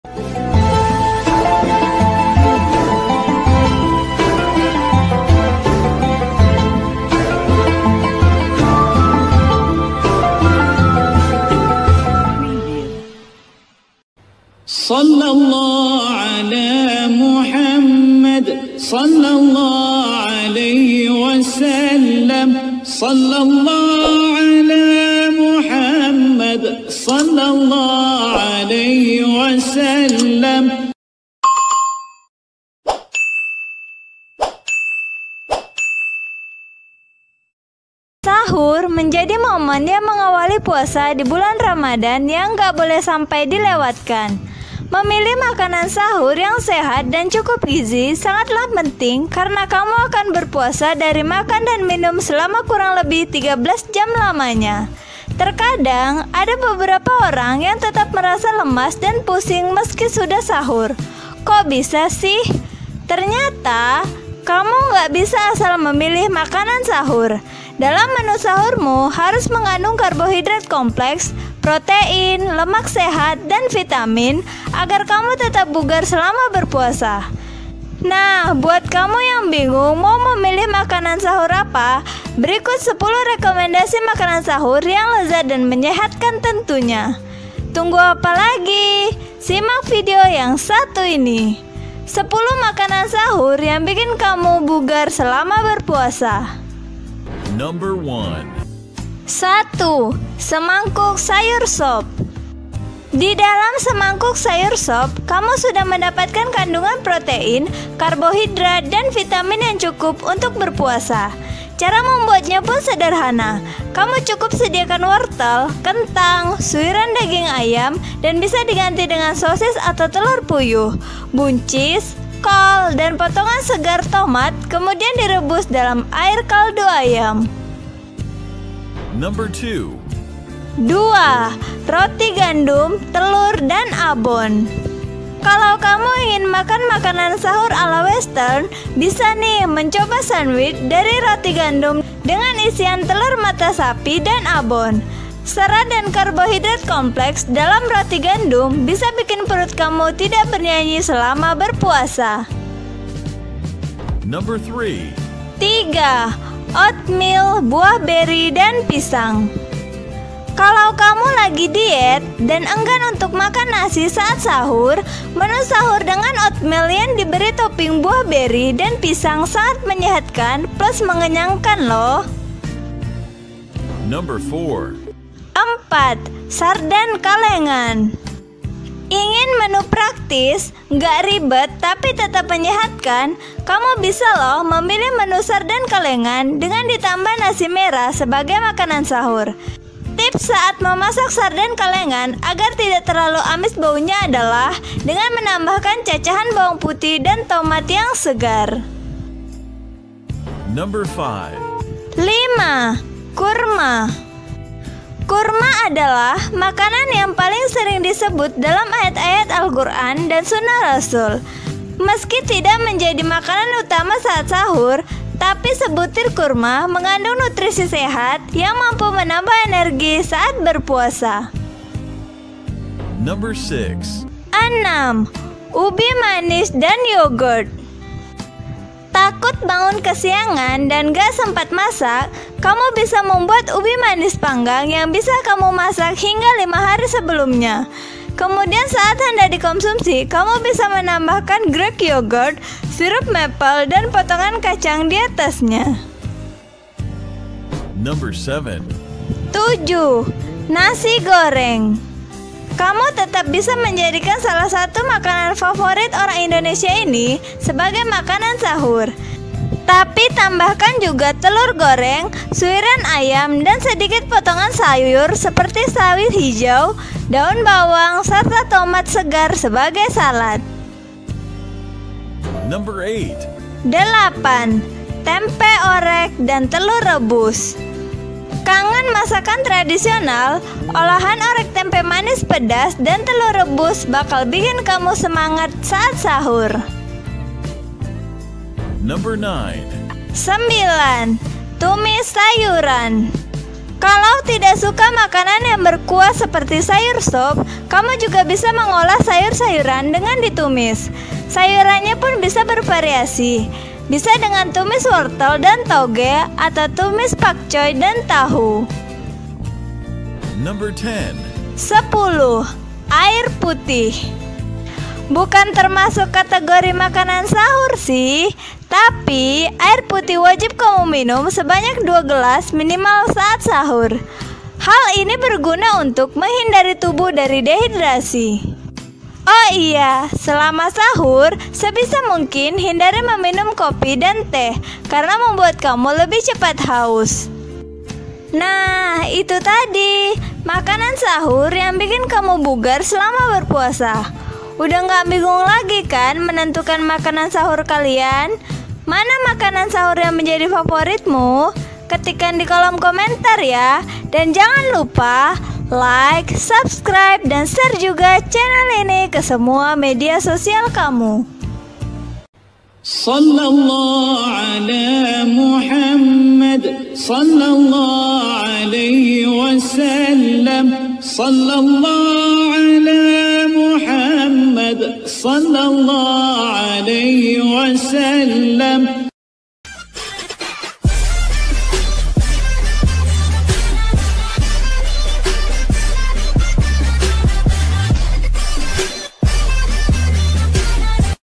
10 MENU SAHUR YANG BIKIN KAMU BUGAR SAAT BERPUASA | MENU SAHUR SEHAT BERGIZI STAMINA PRIMA
Video Ini Berisi Tentang 10 Menu Sahur Yang Bikin Tetap Bugar Selama Berpuasa.

Terima Kasih Kepada Channel YouTube Generasi Millenial.